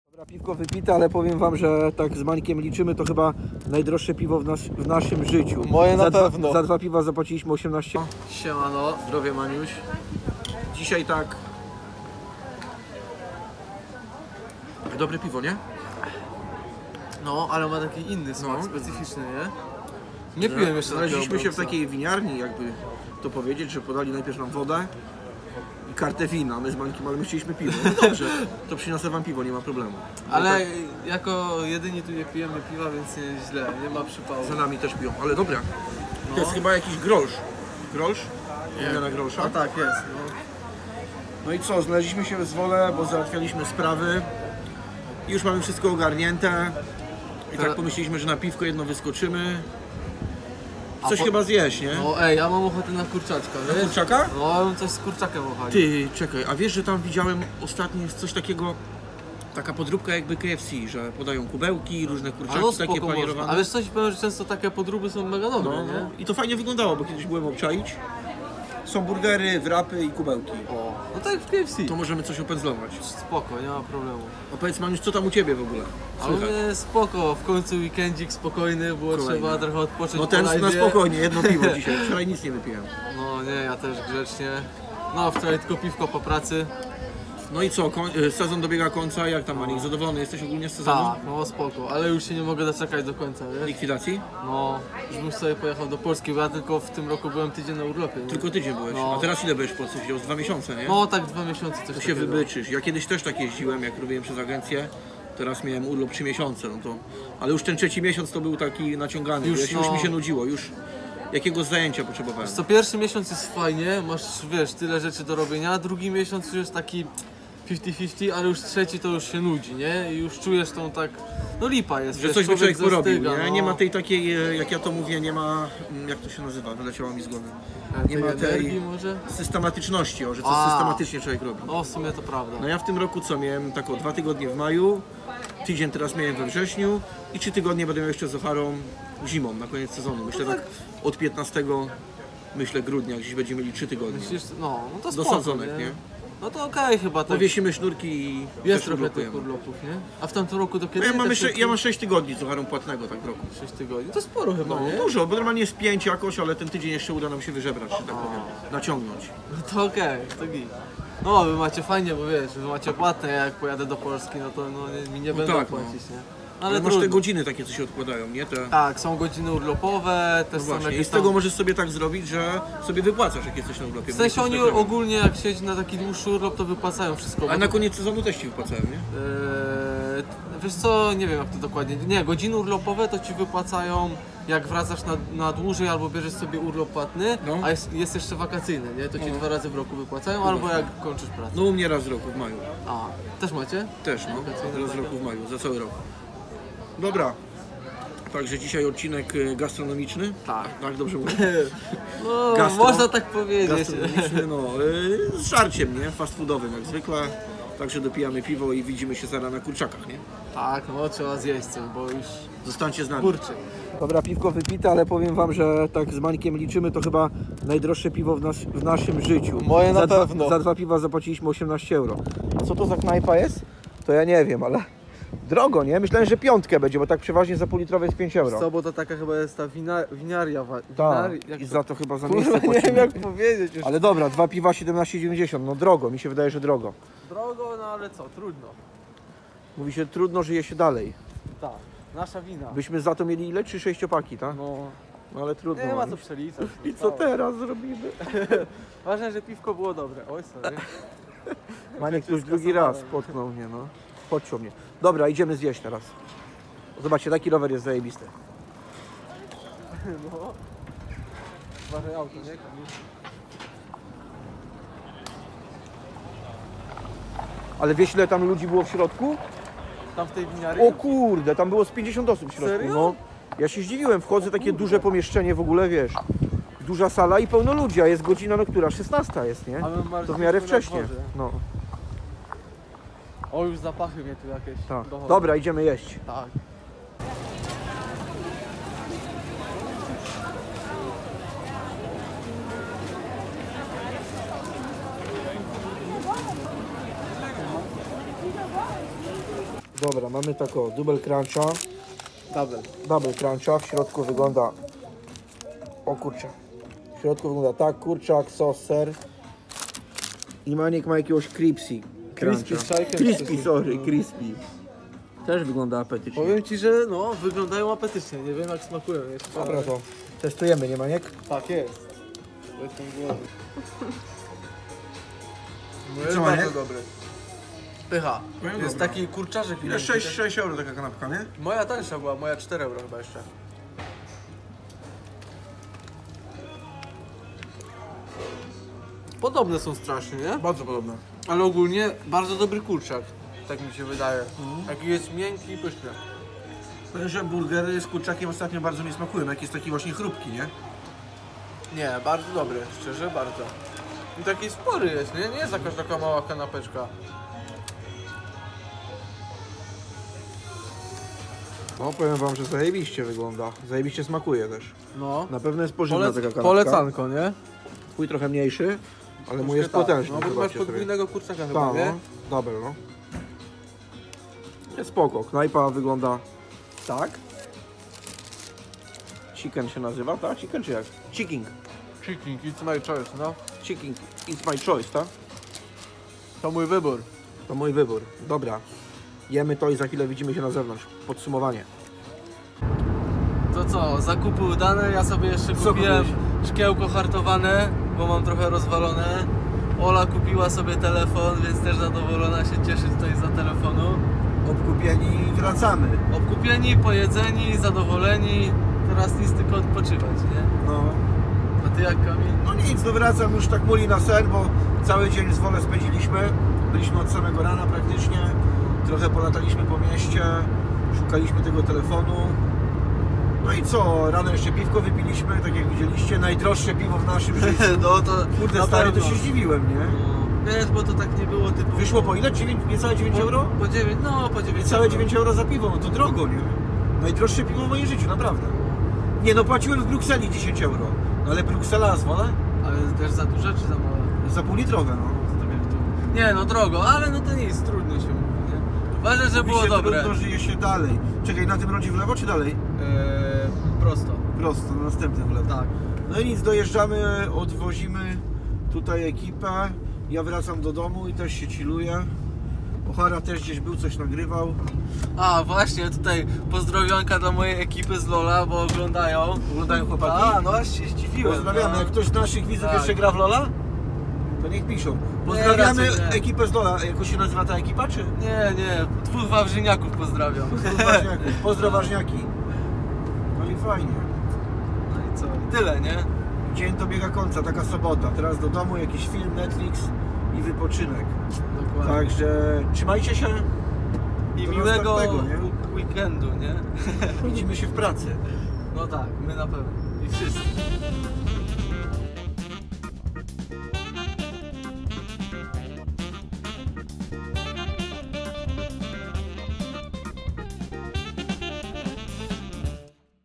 NAJDROŻSZE PIWO JAKIE PIŁEM W HOLANDII / CHICKEN BURGER
09 PAŹDZIENIK 2022 To było najdroższe piwo w moim życiu potem poszliśmy zjeść burgera z kurczakiem.